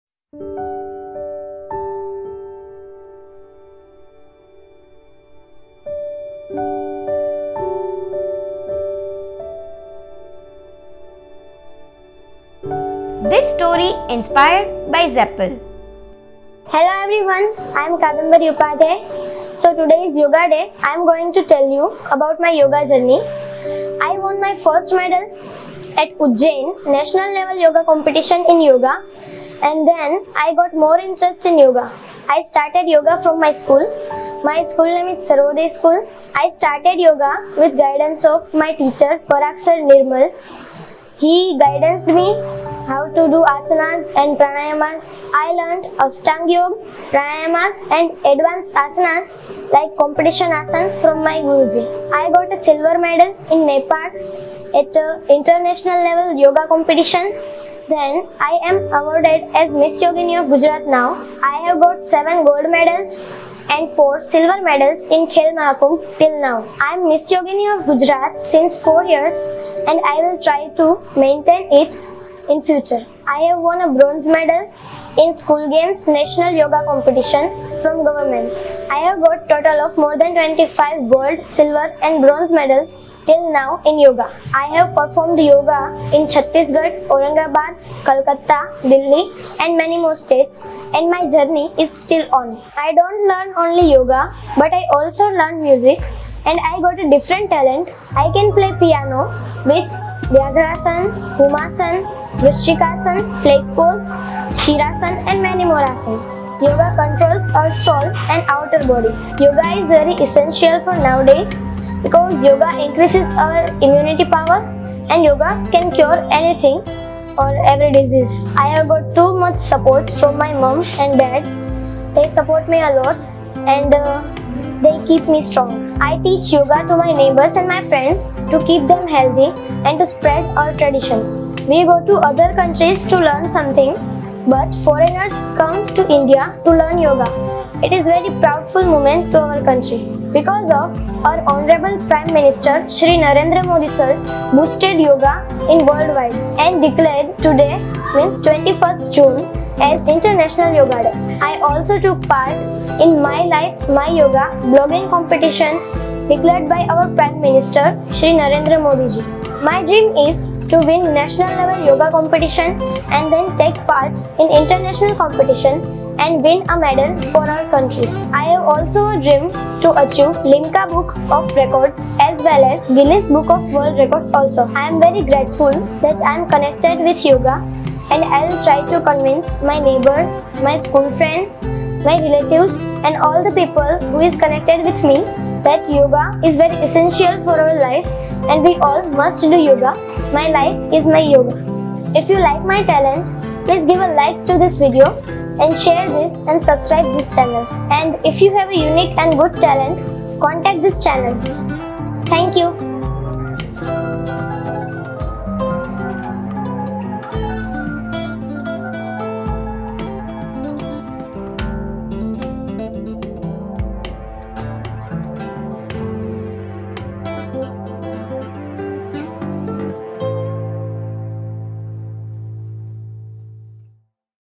Rubberband Girl | International Day of Yoga 2020 | Simple Pranayam Techniques to Family & Friends
રાજકોટમાં સૌથી નાની વયે યોગમાં સફળતા અને કીર્તિ પામનાર કાદમ્બરી ઉપાધ્યાય, માત્ર પાંચ વર્ષની હતી ત્યારથી યોગ કરે  છે.

કંઈ કેટલાય ઇનામો જીત્યા છે શહેર, જિલ્લા લેવલ પર. રાજ્ય કક્ષાએ અને રાષ્ટ્રીય કક્ષાએ સિલ્વર મેડલ પણ રાજકોટની કાદમ્બરી લઇ આવી છે.

"હું સ્કૂલ-કોલેજમાં છોકરીઓને યોગ શીખવું છું, સ્કૂલ જાઉં છું અને બીજી એક્ટિવિટી પણ કરું છું, માત્ર યોગ જ નહિ પણ સંગીત, વક્તૃત્વ અને ડાન્સ સ્પર્ધામાં પણ ઇનામ લાવી છું. શાયદ કોઈ પાસે સ્કૂલથી માંડી ને કોલેજ સુધી જેટલા સર્ટિફિકેટ નહિ હોય એટલા મારી પાસે સ્પર્ધા જીત્યાના સર્ટિફિકેટ્સ છે અને હજી પણ આવશે. યોગ સાધના એ સહેલી નથી હોતી સૌ કોઈ જાણે છે, મોટા મોટા લોકો કલાકમાં તો થાકી જાય છે અને આપણે ગુજરાતીઓ તો શરૂઆતે સુરા એટલે મોટા ઉપાડે શરુ તો કરીએ છીએ પણ 2-3 દિવસ અને વધીને અઠવાડિયામાં સાંજે પાણીપુરીની લારી પર કેહતા જોવા મળીએ કે "ટાઈમ જ ક્યાં મળે છે". દરરોજ ની 2 કલાકની પ્રેક્ટિસ એ પણ એક છોકરી માટે આસાન નથી હોતી. મારી મમ્મી અને પરાગભાઈ નિર્મલ મારા યોગ ગુરુ છે.  એજ મને અલગ અલગ યોગ આસન શીખવે છે. "

"હું તો સૂરજમુખીનું એક નાનકડું ફૂલ,
મને સૂરજ થવાના બહુ કોડ:

સૂરજ ને મળતી આખા નભની વિશાળતા,
મારી પાસે છે એક છોડ"

Visit our world directly: www(DOT)zappl(DOT)in
Lead Producer: Prakash chavda
Production House: Zero Aperture Production PVT. LTD.
Supported: Avnish Gadhvi & ZAPPL team
Special Thanks: Nikhil Bhavsar